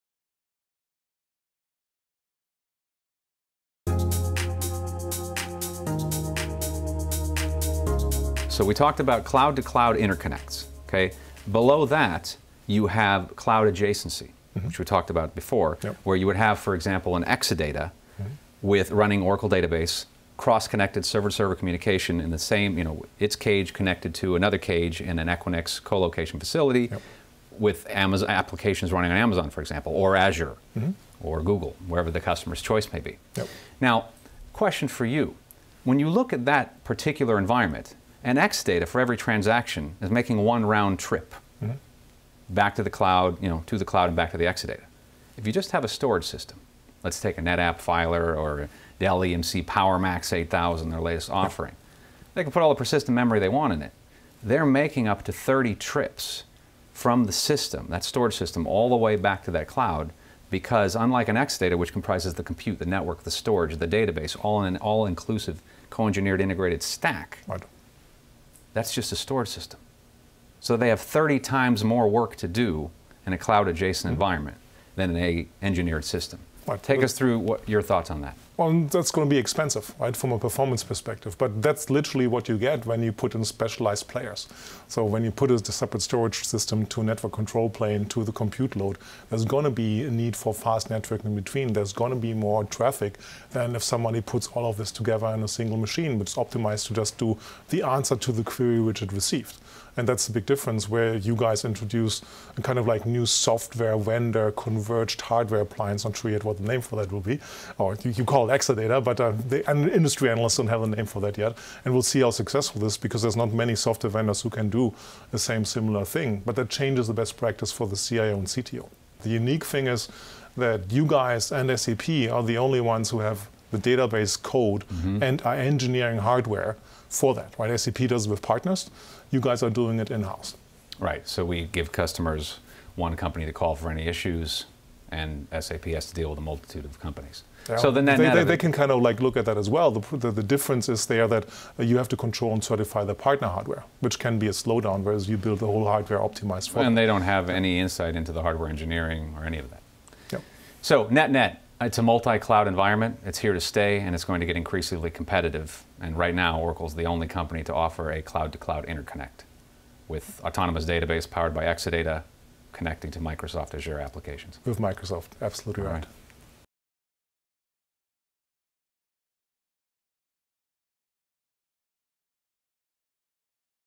Oracle Takes the Lead in Multi-Cloud World—Part 2
Steve Zivanic, VP, Cloud Business Group, Oracle, and Holger Mueller, VP and Principal Analyst, Constellation Research, discuss Exadata's role in cloud adjacency deployments.